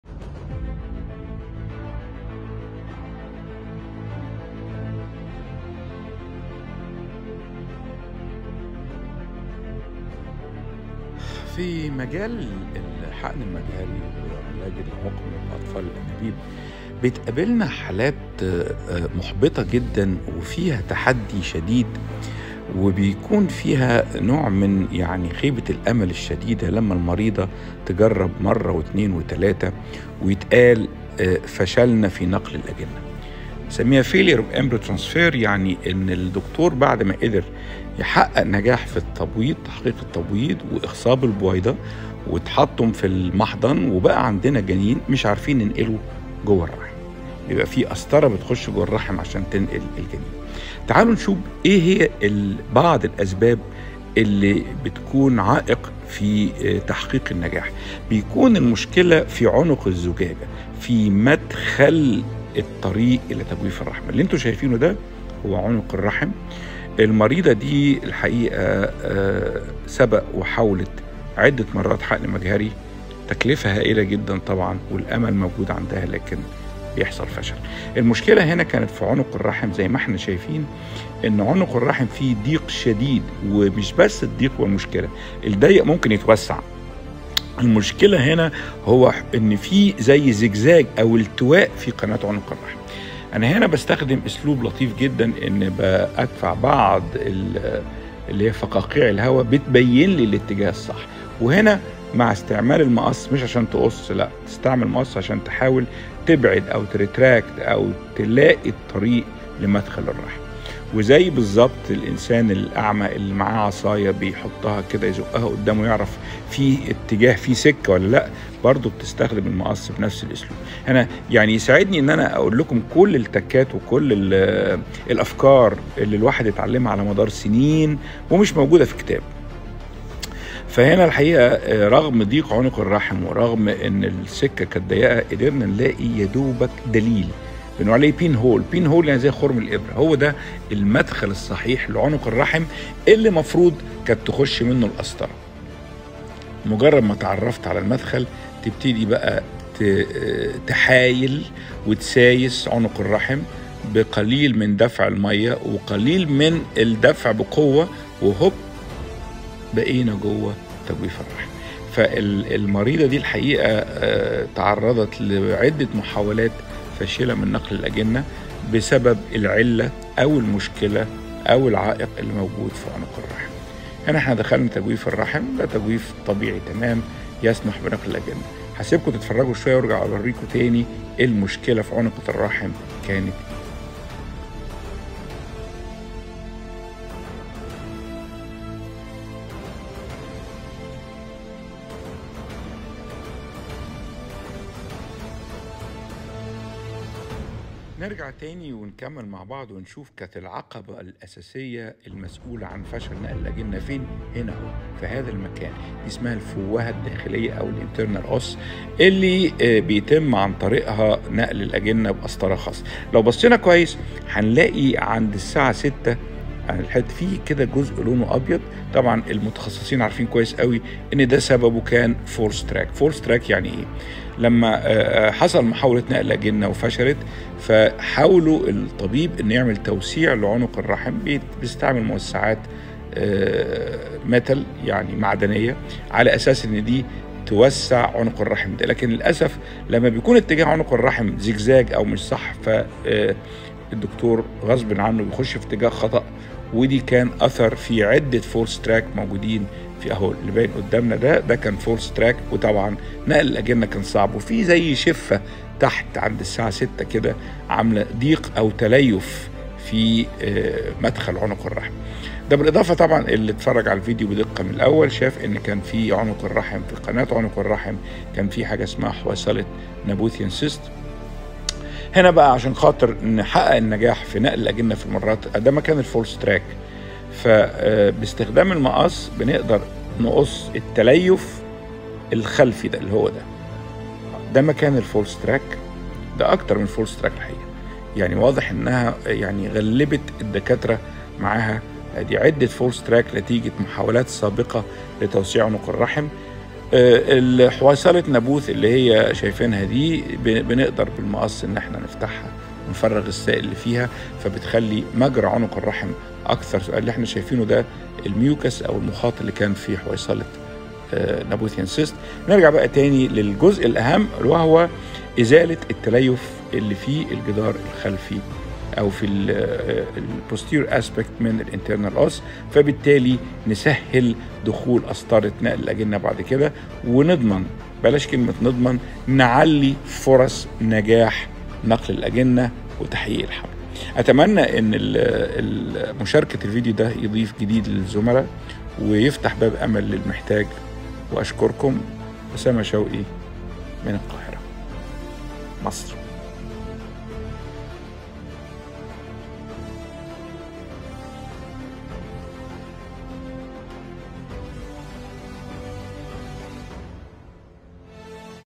الفشل و الامل في تحقيق الحمل في حالات الحقن المجهري، د اسامه شوقي
مشكله عويصه تسبب احباط شديد..

حاله تعاني من عقم اولي مما استلزم اجراء حقن مجهري و نقل الاجنه الي تجويف الرحم..
المريضه اجرت منظار رحم في البدايه ثم بعد ذلك ٤ محاولات حقن مجهري اطفال انابيب  و اكن للاسف بائت كلها بالفشل و لم تنجح..

و تقنيه الحقن المجهري متاحه و لكن مكلفه جدا و قد تصل تكلفه المحاوله الي ٢٥-٣٠ الف جنيه مصري..

طيب نفهم الاول ايه هو بقي الحقن المجهري؟
ببساطه هو اخصاب البويضه في المعمل بدل ما يتم ذلك طبيعيا داخل جسم المرأه..

حيث يتم تنشيط التبويض ب ادويه ذات تكلفه باهظه ، ثم عند اكتمال نضوج البويضات ، يتم التقاطها باسلوب بسيط باستخدام ابره شفط خاصه و ذلك باستخدام الموجات الصوتيه تحت مخدر بسيط..
ثم يتم اخصاب البويضات بخلايا المنويه من الزوج في المعمل تحت تكبير الميكروسكوب..
و بذلك يتم تكوين بذره جنين و يتم حفظها في بيئه خاصه و محضن ذو تقنيه عاليه حتي يتم انقسام الخلايا و تصبح جاهزه للنقل داخل الرحم..
بعد ذلك يتم نقل الاجنه الي داخل الرحم بواسطه قسطره بلاستيكيه مرنه..

في الحقيقه ان نجاح عمليه الحقن المجهري يعتمد علي عده عوامل لكن بلا شك اهمها هو القدره علي توصيل الاجنه الي المكان المناسب داخل الرحم..

في بعض الحالات مثل الحاله التي امامنا ، كان هناك صعوبه شديده في دخول القسطره من خلال عنق الرحم ..
و حاول الاطباء محاولات مضنيه للتغلب علي عقبه عنق الرحم لكن كل المحاولات ذهبت هباء..
تم اعاده منظار رحم ٣ مرات و للاسف لم يستطيع دخول الرحم ..

الفيديو يوضح وجود التواءات شديده في عنق الرحم و ان محاولات منظار الرحم الاولي تسببت في مسار خاطئ و حدوث تليف حاد في نسيج عنق الرحم و ايضا وجود شفه متليفه internal os at 6 oclock position , اغلقت تماما مدخل الرحم..

في هذا الفيديو قمت باستكشاف مسار عنق الرحم بحرص شديد و منتهي الدقه و استخدام المقص بحذر لتحديد الاتجاه الصحيح لمدخل الرحم..
و تم قص التليف الموجود في الحزء السفلي من فوهه الرحم الداخليه internal os
و ايضا فتح و ازاله حويصله نابوث في قناه عنق الرحم و التي تعيق دخول قسطره نقل الاجنه..
و بعد ذلك تم دخول تجويف الرحم بمنتهي اليسر و السهوله..
ثم تم توسيع عنق الرحم الي مستوي مناسب يسمح بنقل الاجنه لاحقا..

ثم كررت المريضه محاوله اخيره للحقن المجهري بعد المنظار بشهرين و هي حاليا حامل في توأم الحمد لله بعد ٤ محاولات فشل حقن مجهري،  ..

منظار الرحم سلاح رائع و خطير..
يقدم فائده عظمي لمن يستخدمه باسلوب صحيح و واعي ، و قد يتسبب في كارثه لو تم الاستهانه و استسهال استخدامه..
و دمتم بخير و علم و حكمه..
اسامه شوقي
استاذ بكليه طب قصر العيني